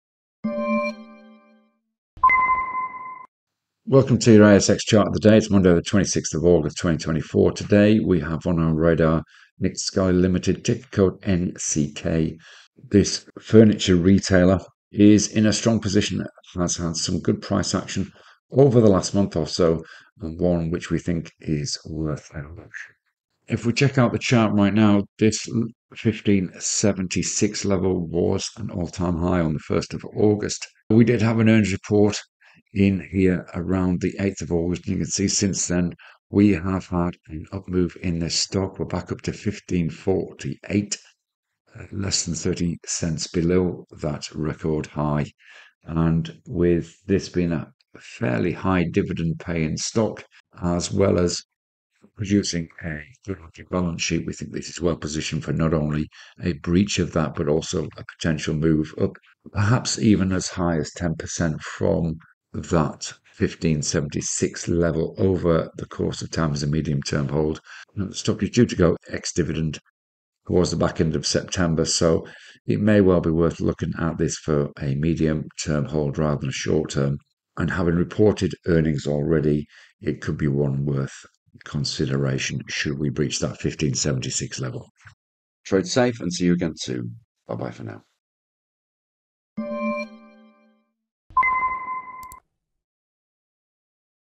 ASX Chart of the day- NCK - Furniture retailer technically poised for a breach of previous high
Nick Scali Limited (NCK) is engaged in sourcing and retailing of household furniture and related accessories. Nick Scali sources its products from around the world and imports directly from manufacturers globally. NCK operates in Australia and New Zealand.